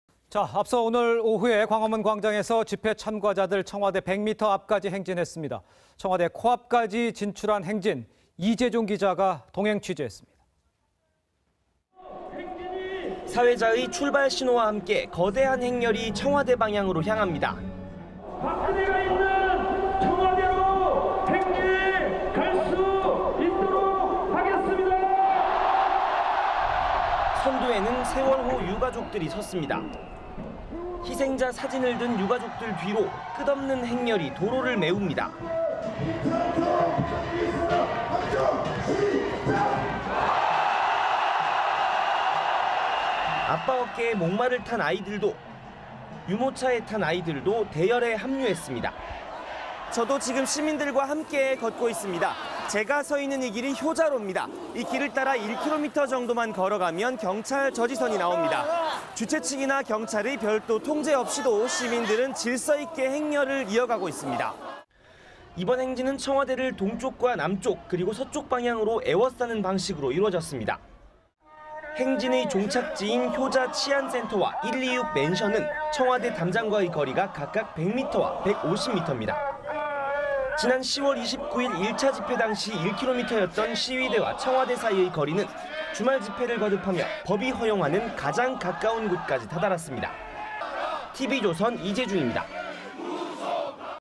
청와대 앞 100미터…청와대 코앞 다다른 시민들
앞서 오늘 오후엔 광화문 광장에 집회 참가자들은 청와대 100미터 앞까지 행진했습니다.